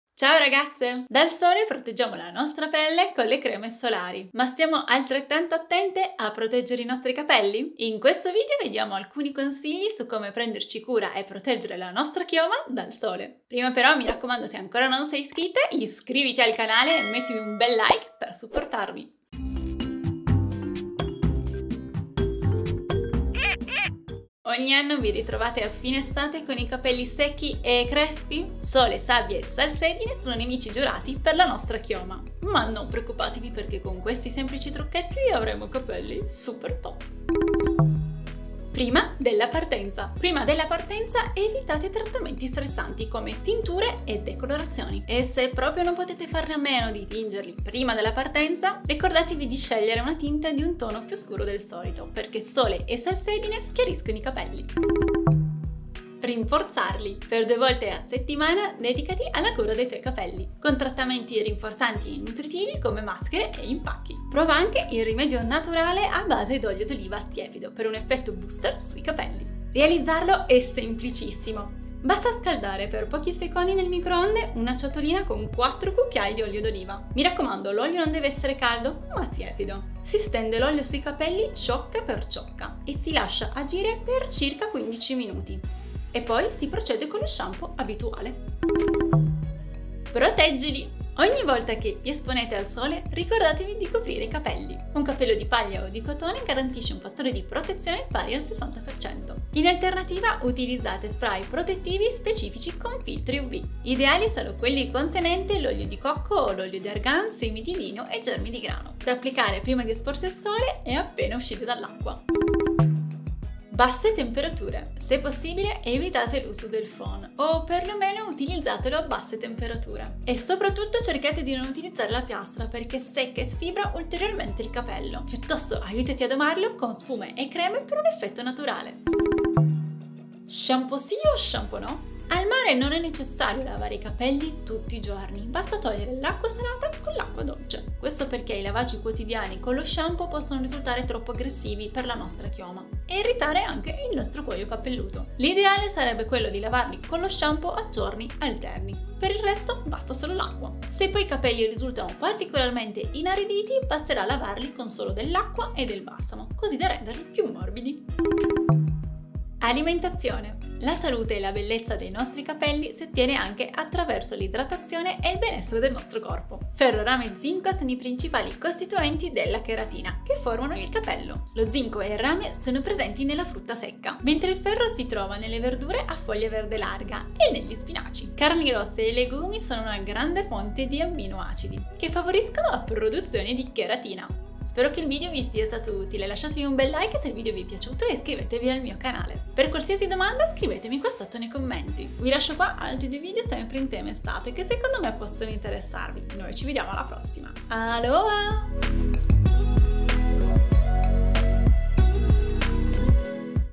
Come fare per PROTEGGERE I CAPELLI DAL SOLE 🌞😉
Non sai come proteggere i capelli dal sole? Ecco i trucchetti da fare per PRENDERSI CURA DEI CAPELLI e gli errori da evitare per non rovinare i capelli d'estate. Sei pronta a dire bye bye alle doppie punte e a sfoggiare CAPELLI LUNGHI da favola💖